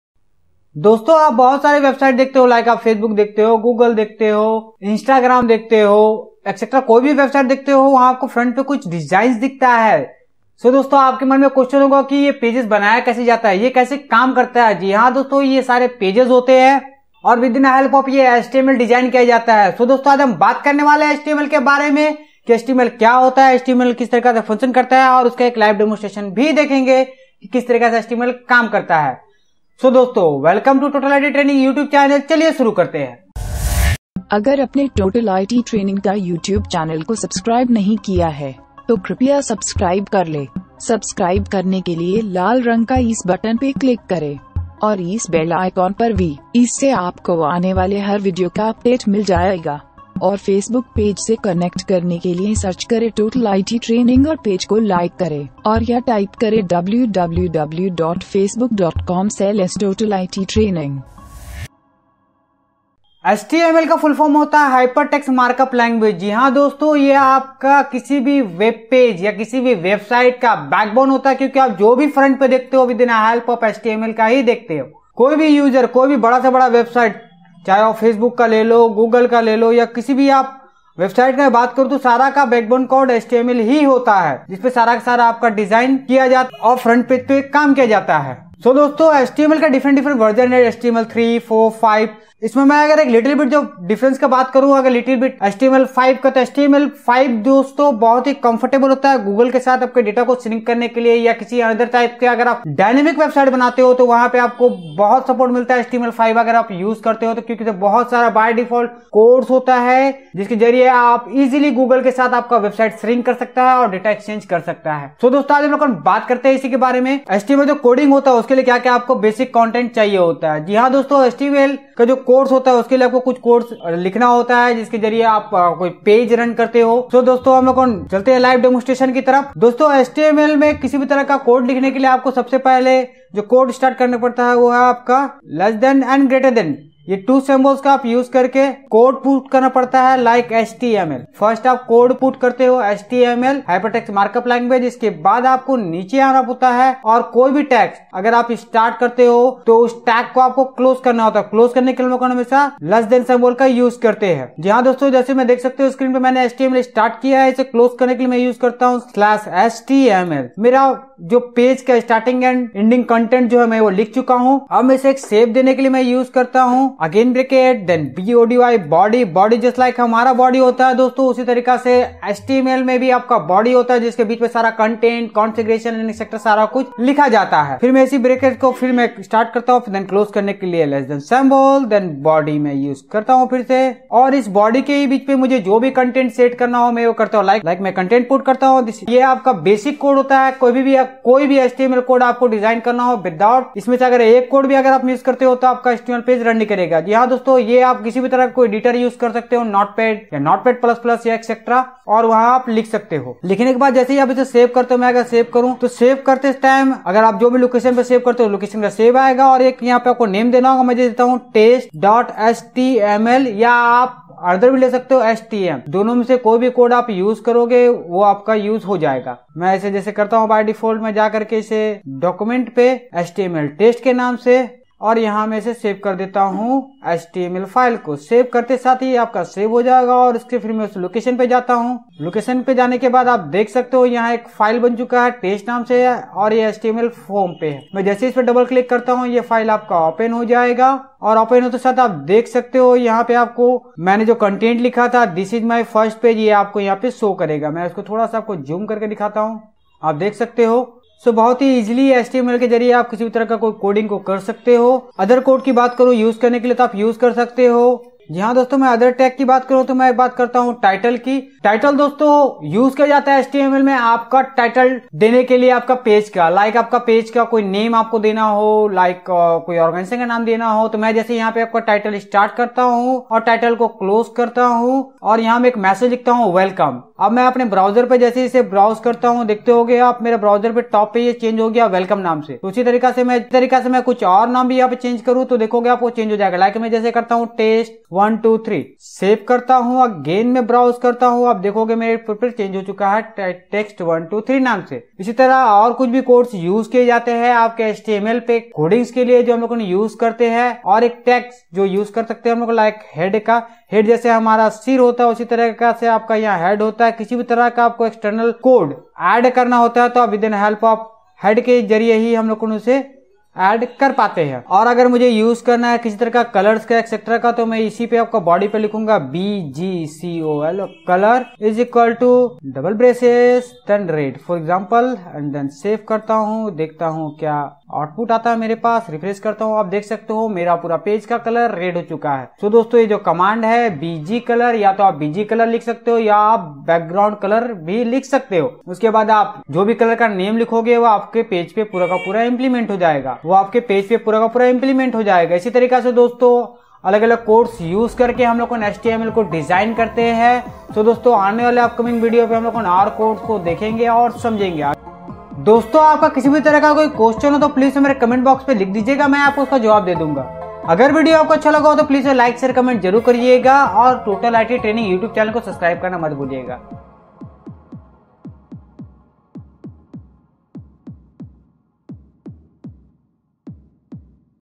What Is HTML Full Introduction in Hindi for beginner By Total IT Training
What Is HTML ||Total IT Training || HOST BY  VIVEK SHARMA || 2018
1) What is HTML in Hindi
2) Why to use HTML ?
3) Difference between HTML, CSS, JS (Live Demonstration in Hindi)
4) History of HTML in Hindi
5) Stable version of HTML
6) Html Color BG

Total IT Training Provider all Types of Courses like ccna mesc hardware networking web designing etc